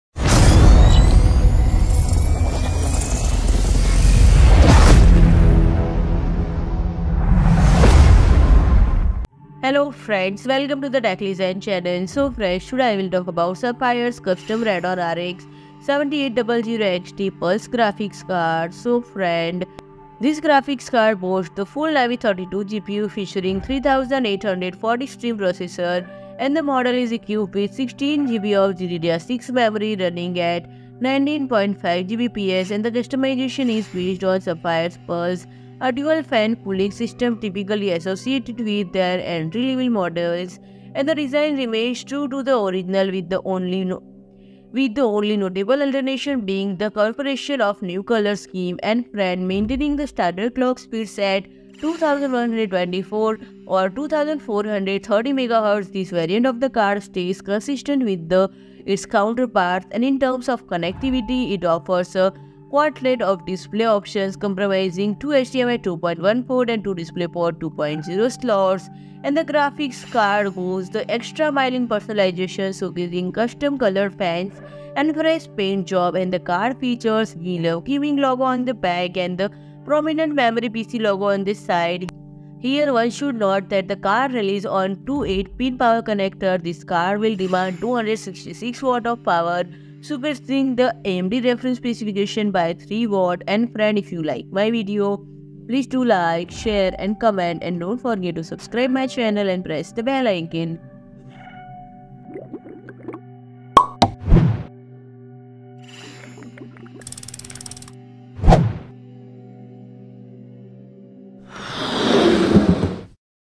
Sapphire PULSE Radeon RX 7800 XT Gaming Memory PC Edition Launched - Explained All Spec, Features
PRODUCT BRAND STORE AMAZON

SAPPHIRE PULSE AMD RADEON™ RX 7800 XT GAMING MEMORY PC EDITION represents a new era in the PC gaming world. It sets new standards with its striking Memory PC design and impressive graphics performance. The powerful AMD RDNA™ architecture, combined with cutting-edge technologies, allows you to enjoy gaming at the highest quality and at unmatched speed. STRIKING APPEARANCE DESIGNED FOR GAMERS, RECOMMENDED BY EXPERTS Are you ready for a breathtaking gaming experience? Then let yourself be inspired by the SAPPHIRE PULSE AMD RADEON™ RX 7800 XT GAMING MEMORY PC EDITION - a masterpiece that was developed for gamers who don't want to make compromises. Upgrade now to the SAPPHIRE PULSE AMD RADEON™ RX 7800 XT GAMING MEMORY PC EDITION and experience gaming in a new dimension. Your games will never be the same again.